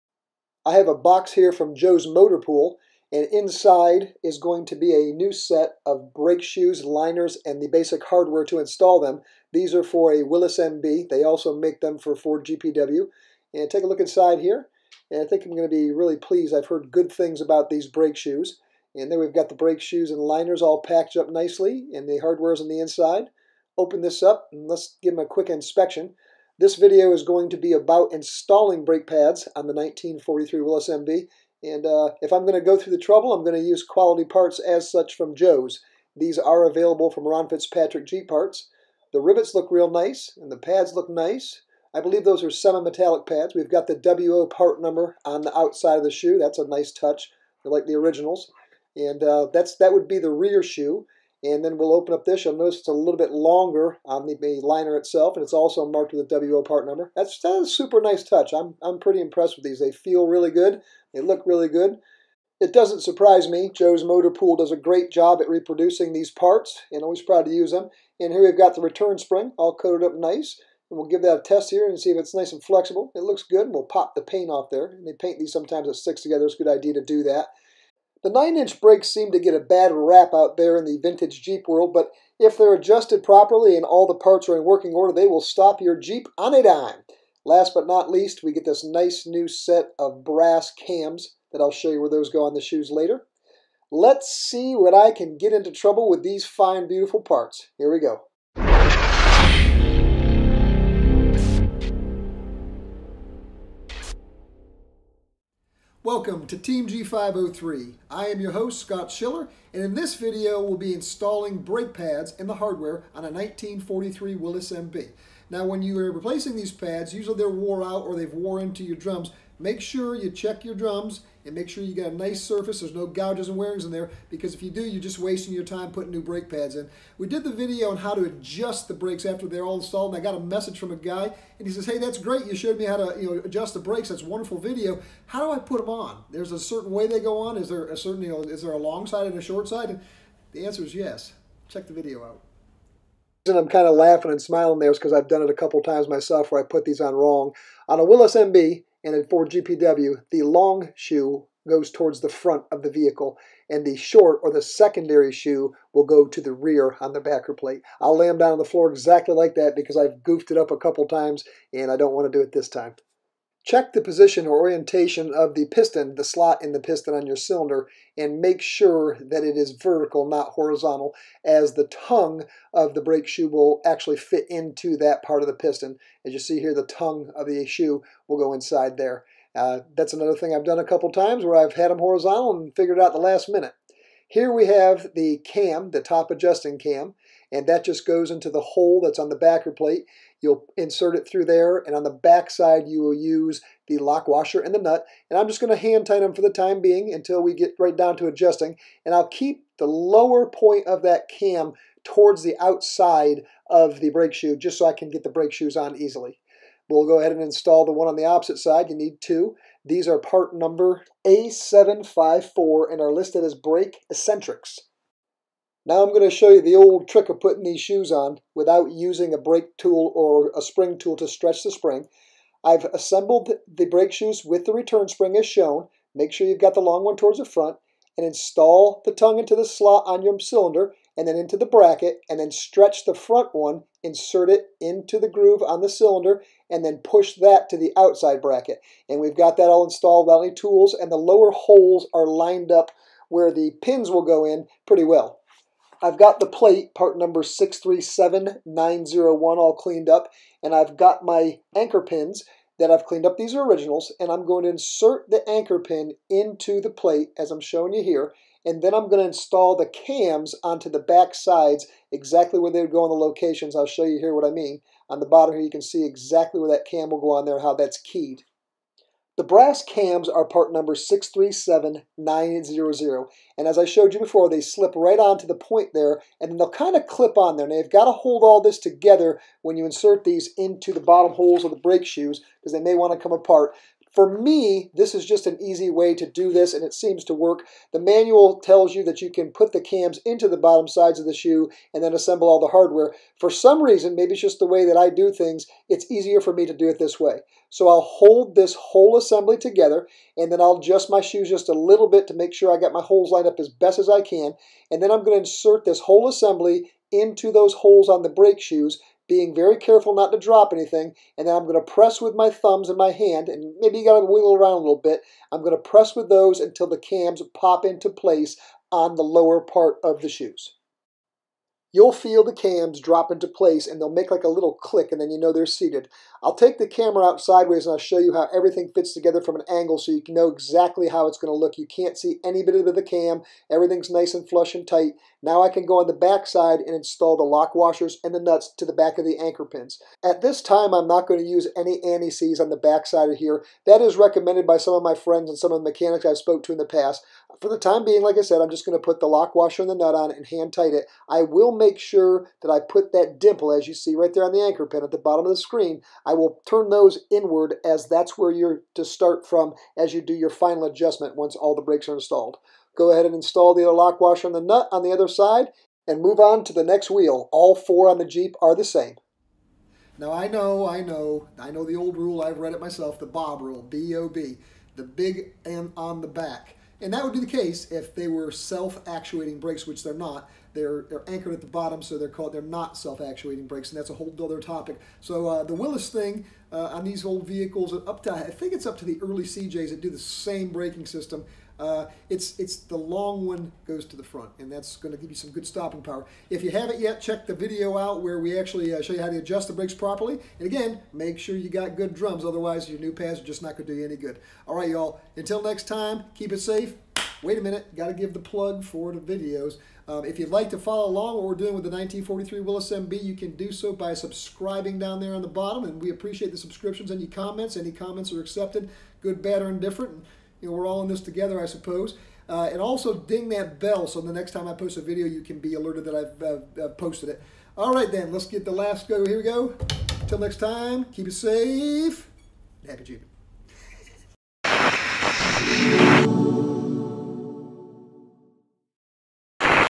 Installing The Brake Shoes And Hardware On A Willys MB Team G503
Video showing the installation of the brake shoes and hardware on a 1943 Willys MB. Part of the Team G503 / G503 TV Series on Youtube. All parts provided by Ron Fitzpatrick Jeep Parts.